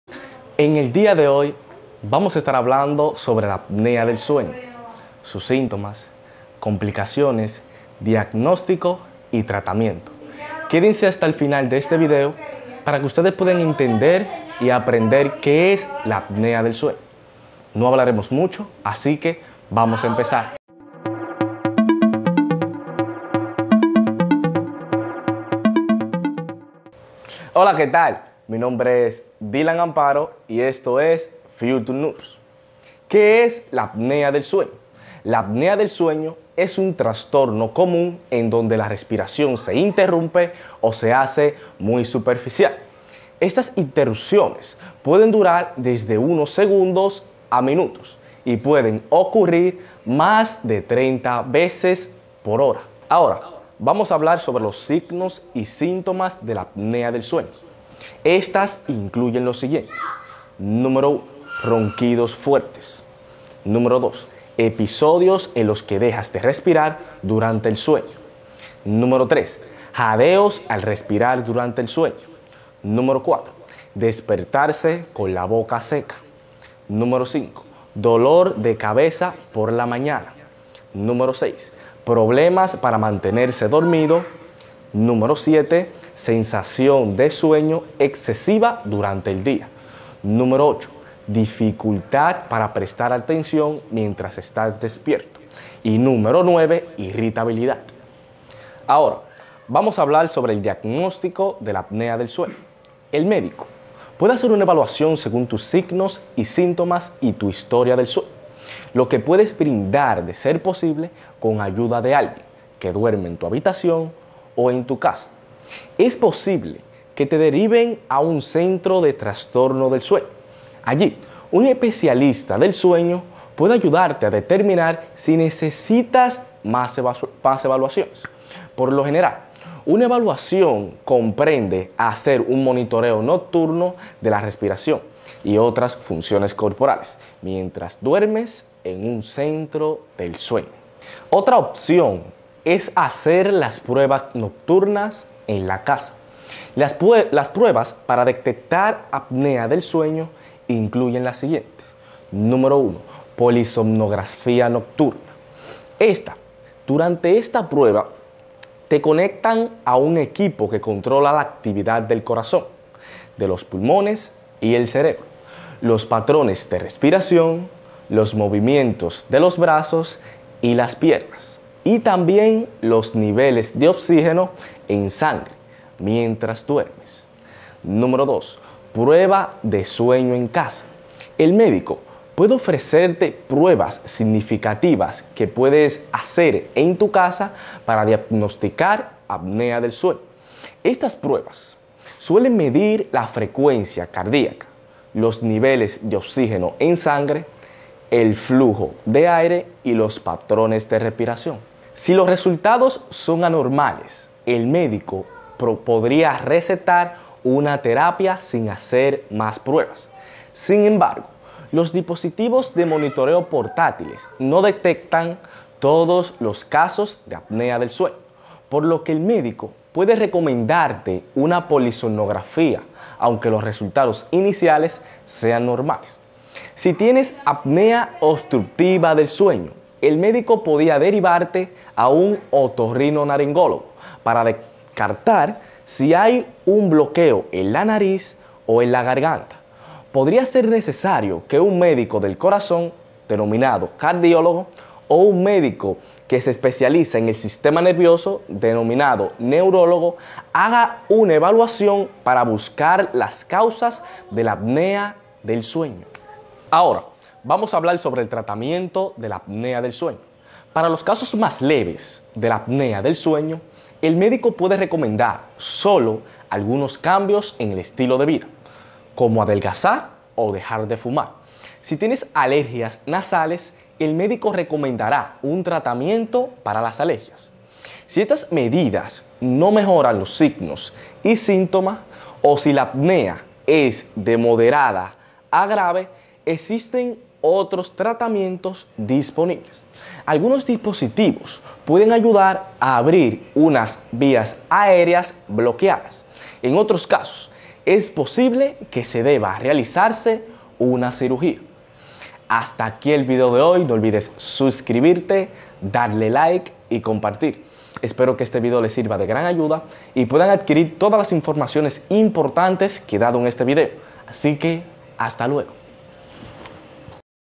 😴Apnea obstructiva del sueño: TRATAMIENTO✨
😴Aprende de una Forma FACIL y SENCILLA sobre la APNEA OBSTRUCTIVA DEL SUEÑO y su TRATAMIENTO IMPORTANTE.✔ MÍRALO Y APRENDE👀

✔para TRATAR la APNEA OBSTRUCTIVA DEL SUEÑO👀 La APNEA OBSTRUCTIVA DEL SUEÑO se trata normalmente mediante terapia CPAP. Durante la terapia, un dispositivo CPAP envía aire presurizado a través de una mascarilla CPAP para mantener sus vías aéreas abiertas mientras duerme.😴

✨Quédate hasta el FINAL de este video y serás CAPAZ de TRATAR la APNEA OBSTRUCTIVA DEL SUEÑO✔